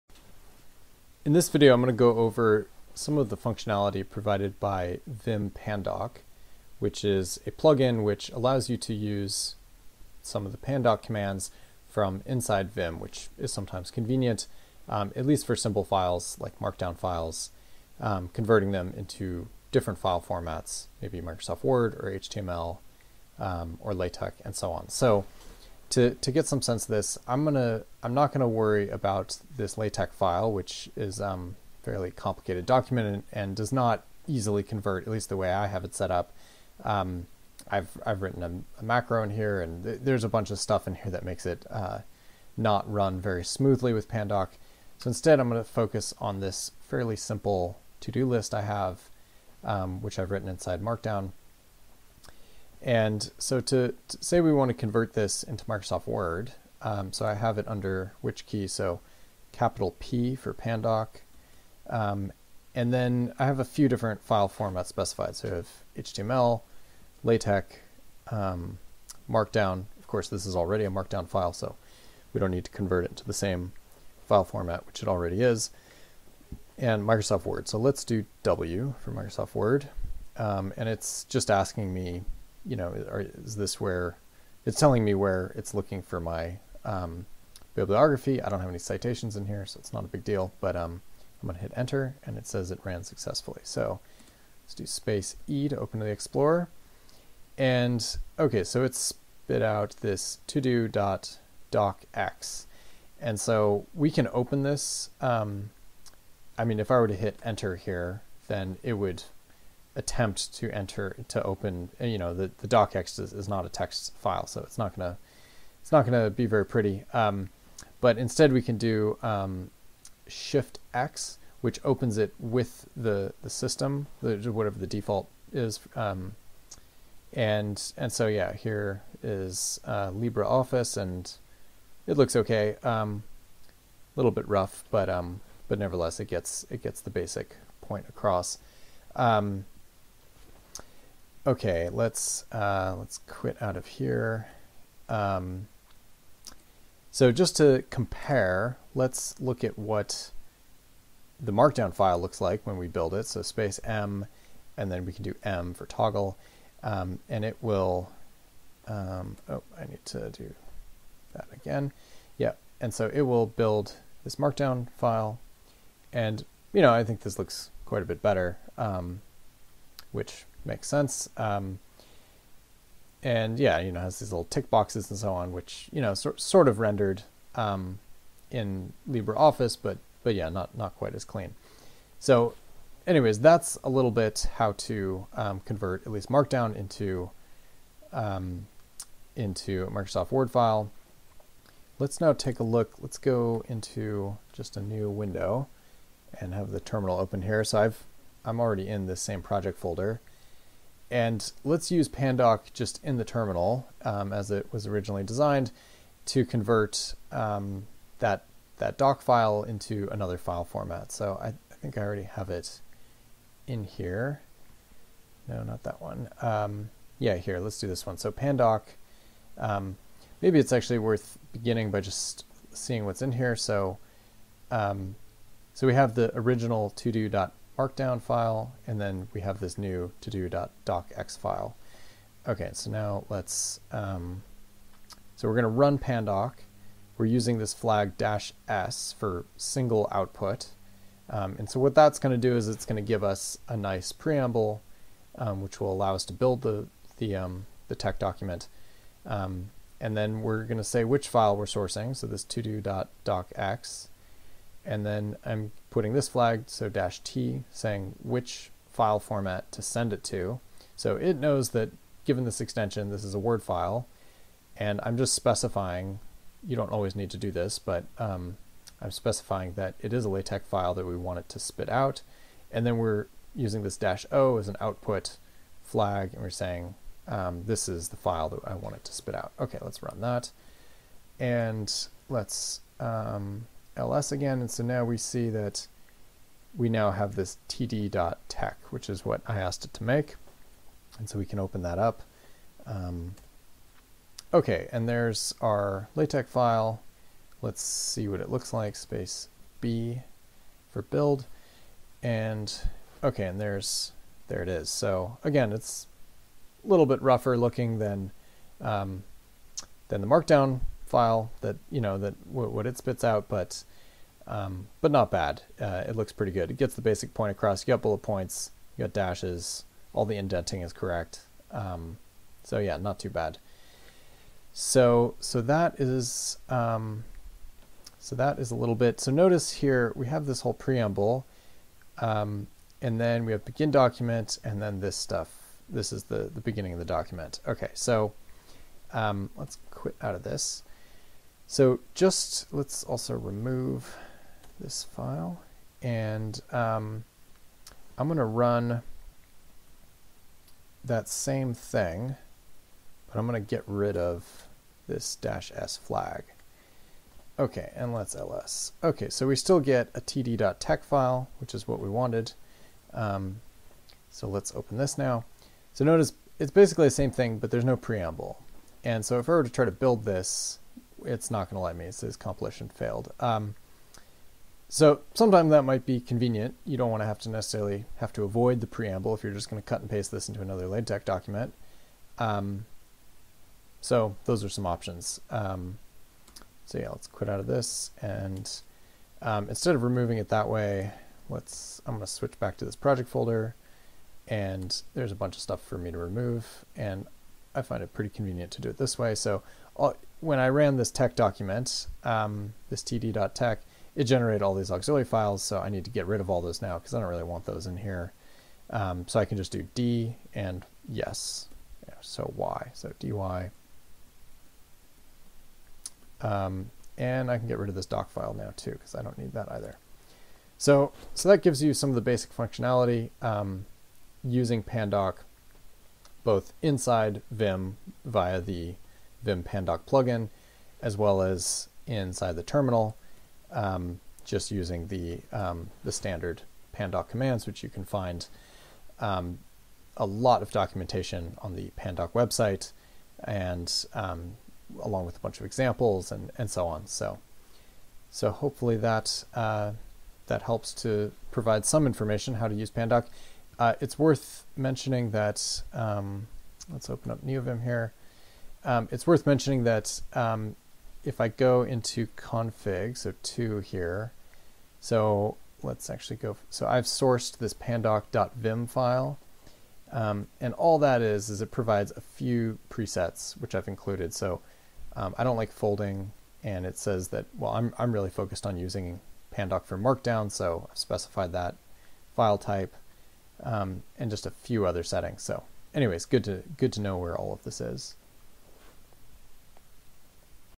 Demo Part 7: Pandoc Plugin for Converting Between LaTeX, Markdown, Word, etc. in Neo Vim
In this video I review the basic functionality provided by Pandoc to convert between different file types both inside NeoVim as well as in the terminal.

You can find all of my configuration files on my GitHub repository, along with installation instructions in the following video: